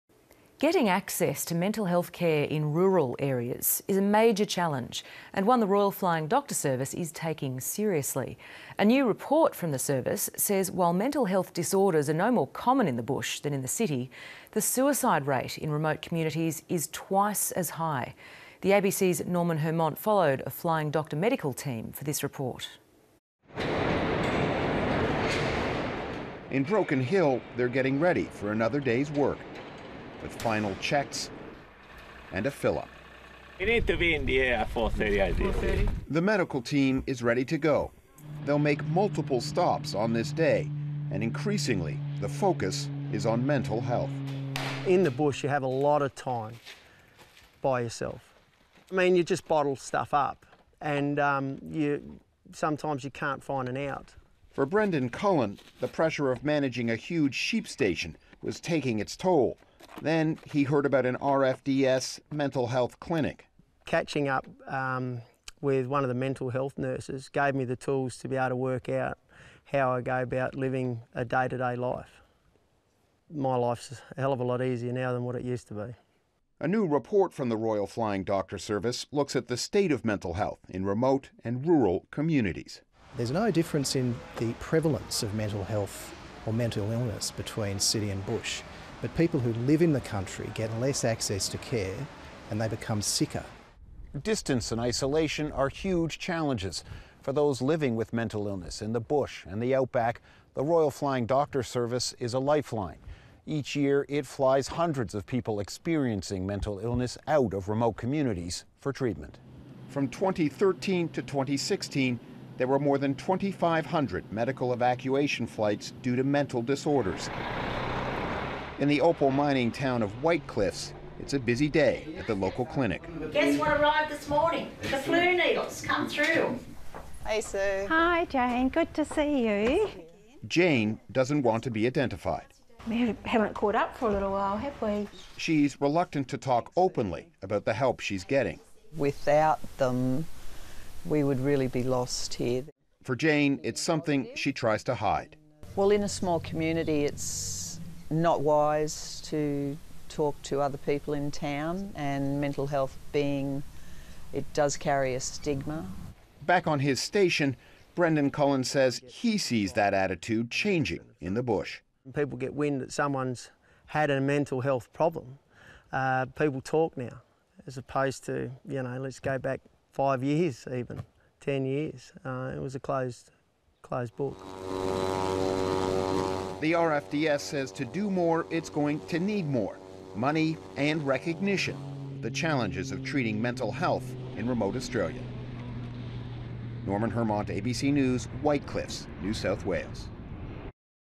Bush Mental Health ABC TV
Mental health disorders are not more common in rural and regional Australia than they are in Australia's cities, according to a new report from the Royal Flying Doctor Service. But they are a lot harder to treat.